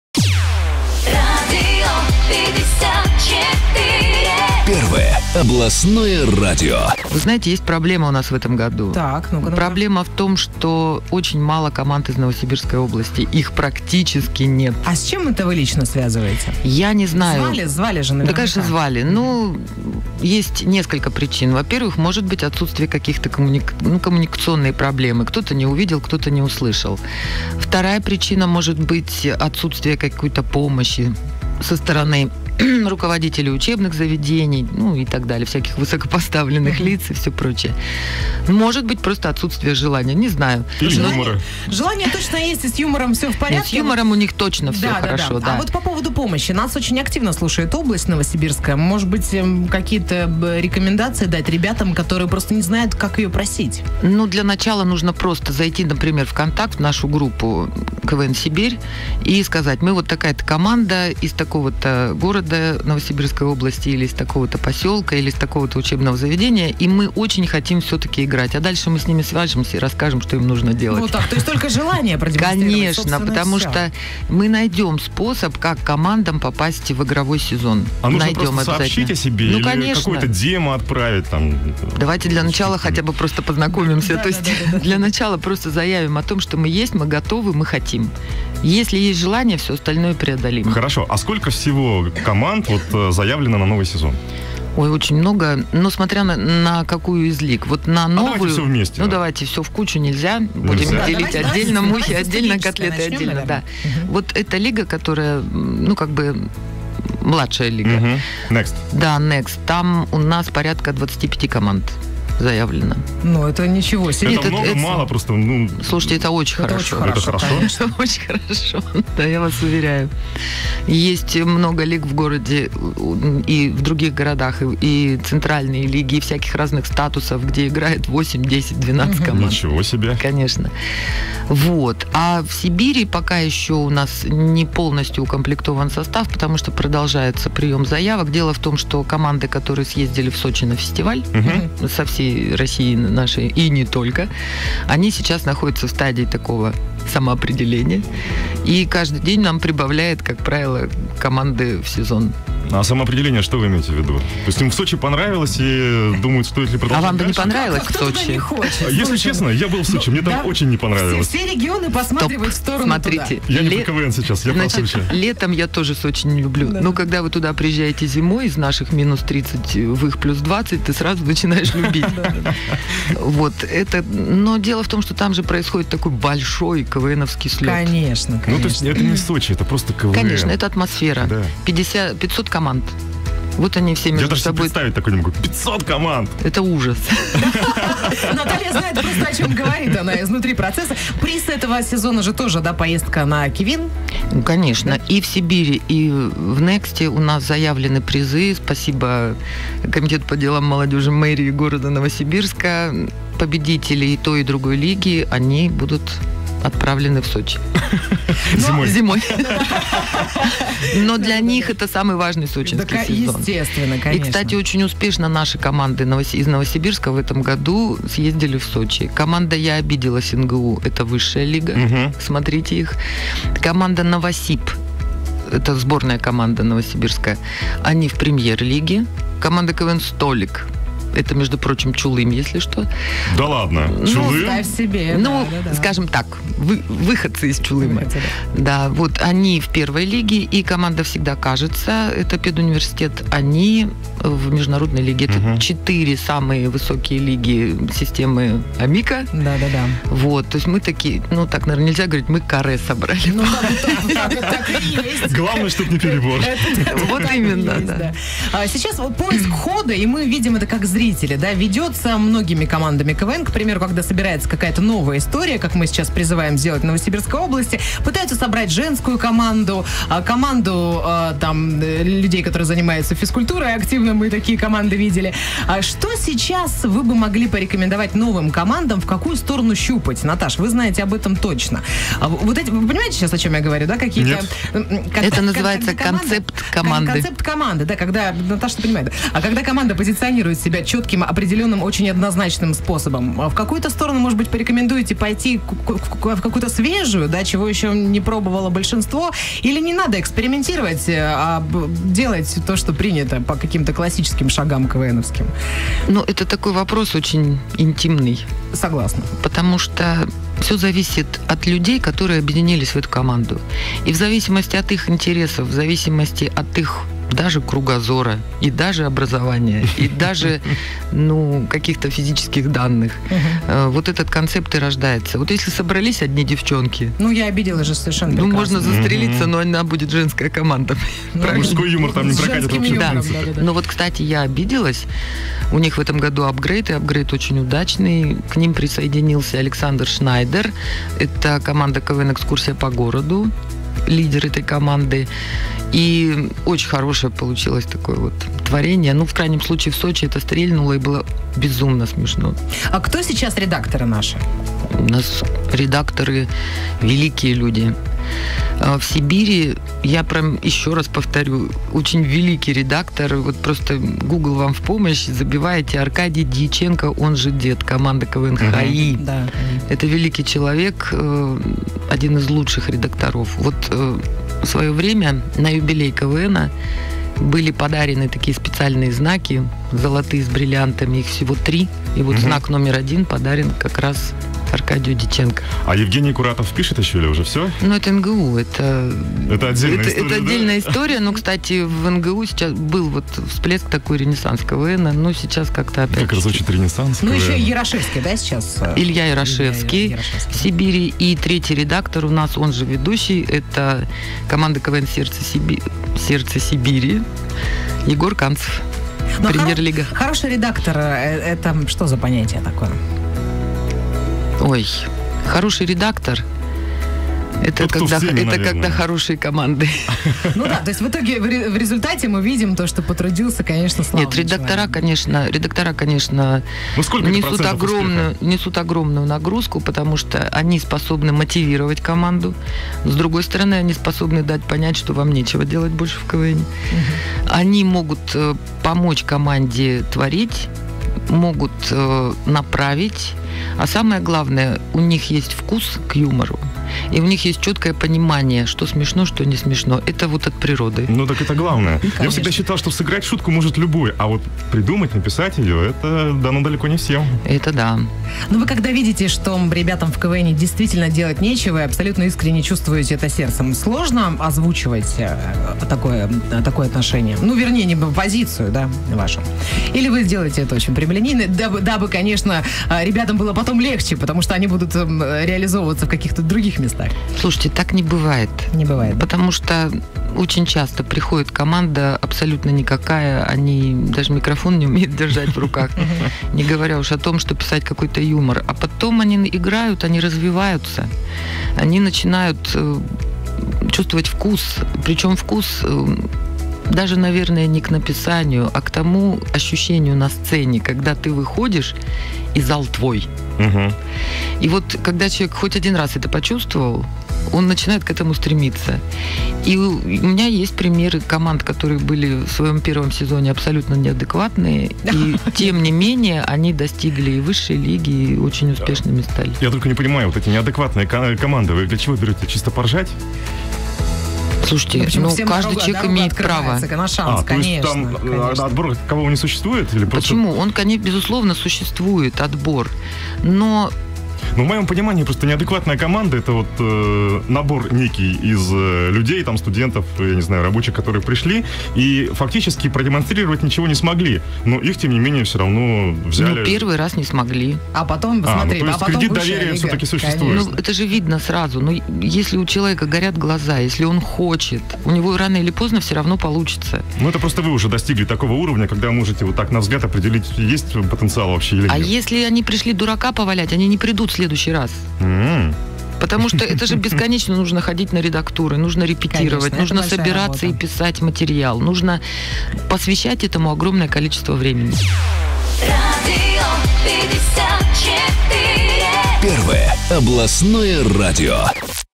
Директор межрегиональной лиги "КВН-Сибирь" Наталья ГАРИНСОН
📢 19 ФЕВРАЛЯ в утреннем шоу ВСЕСВОИ - директор межрегиональной лиги "КВН-Сибирь" Наталья ГАРИНСОН.

👉🏻 Официальная лига «КВН-СИБИРЬ» «КВН-Сибирь» готовится к новому сезону 2020 года. Отборочная игра пройдёт 21 февраля на сцене ДК Горького. Начало в 18:00.